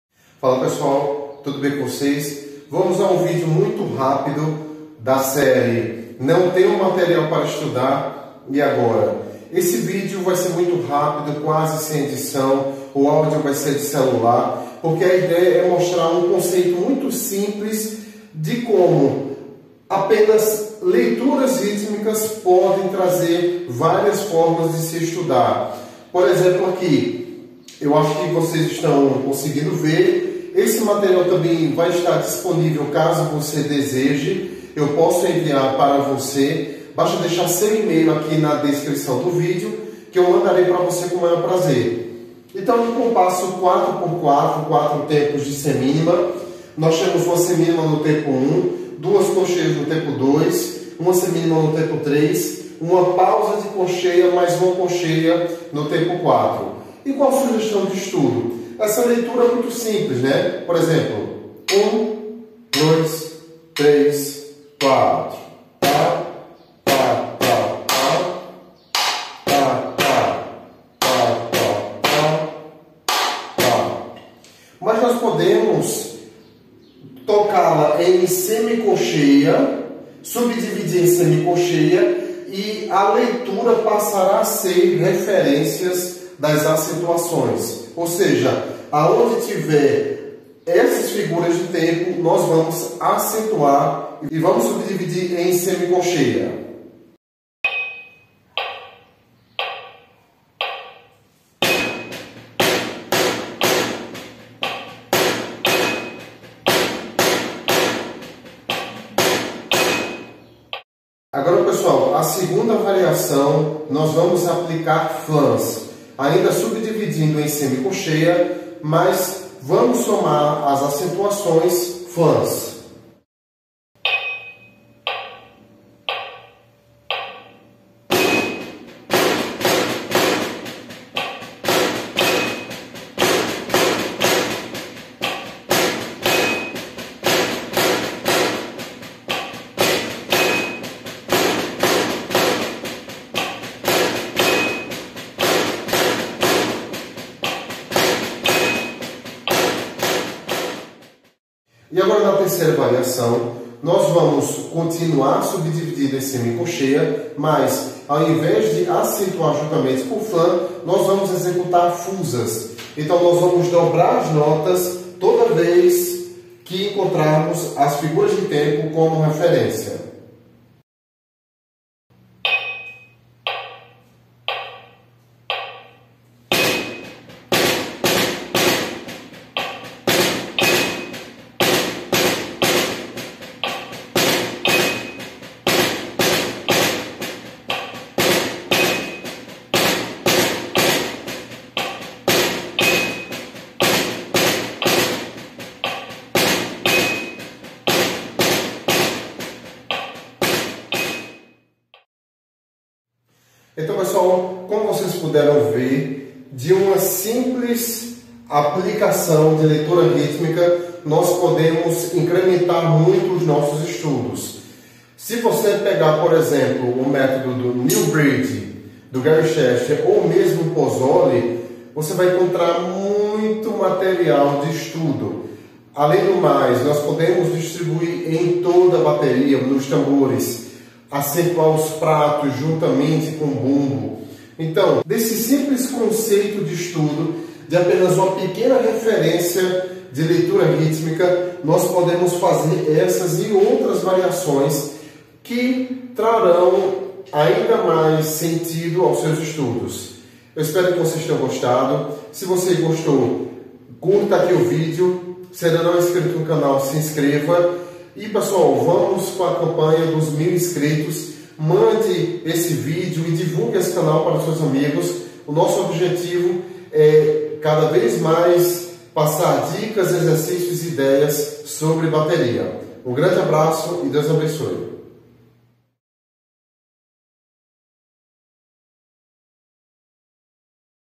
Sugestão de criação de estudos por meio de leituras rítmicas.
Neste vídeo, falo de maneira muito objetiva, como você pode criar seus próprios estudos, por meio de leituras rítmicas, além da coordenção e precisão, também ajudará na criatividade.